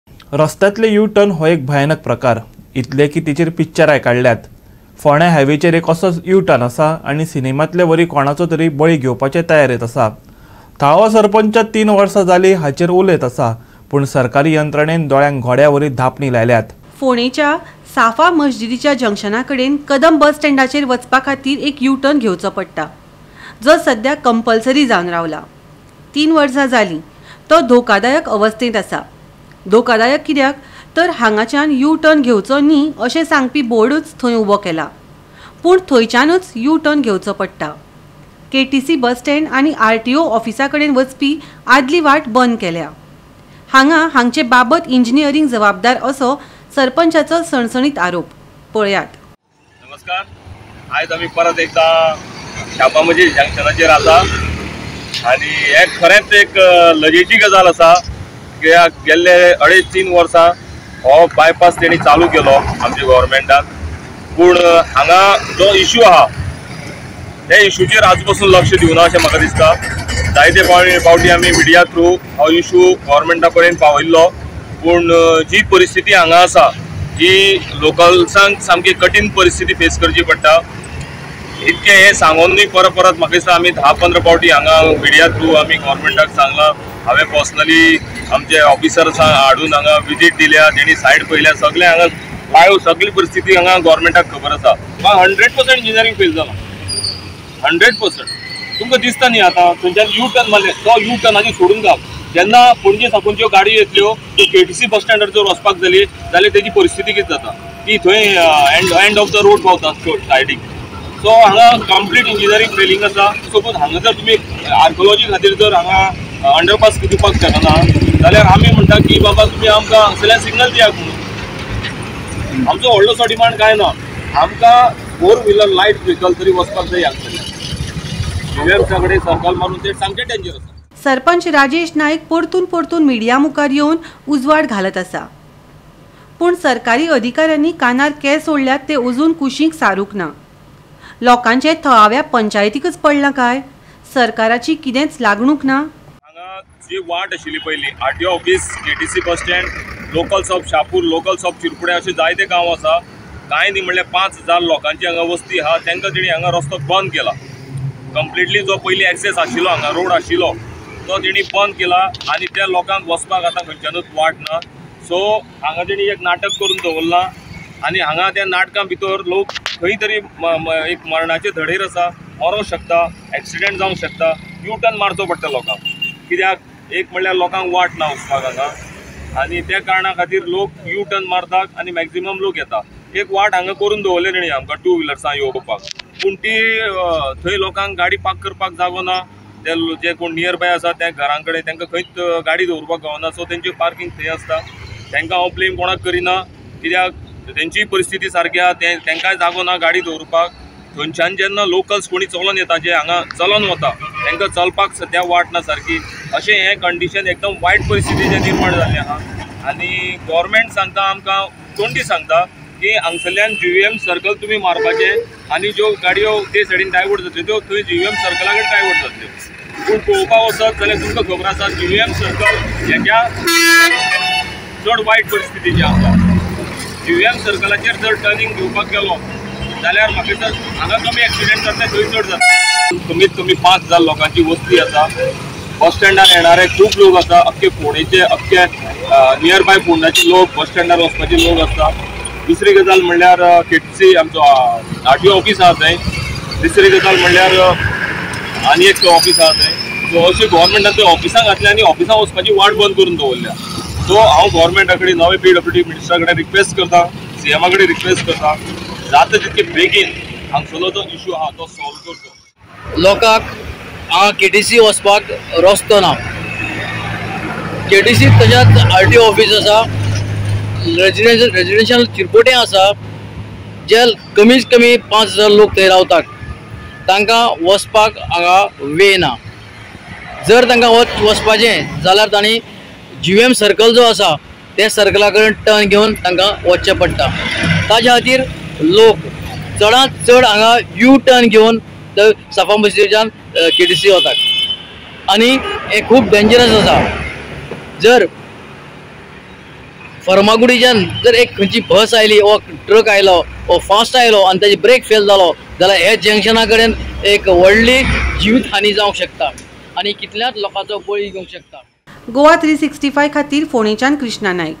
फोंडेंचो यू टर्न भयानक....! बळी घेतलो अचानक…!!
रस्त्यांतले यू टर्न हो एक भयानक प्रकार. इतलो की तेचेर पिक्चरांय काडल्यांत. फोंड्यांय हायवेचेर एक असोच यू टर्न आसा आनी सिनेमांतलेवरी कोणाचो तरी बळी घेवपाचे तयारेंत आसा. थळावो सरपंच तीन वर्सां जालीं हाचेर उलयत आसा,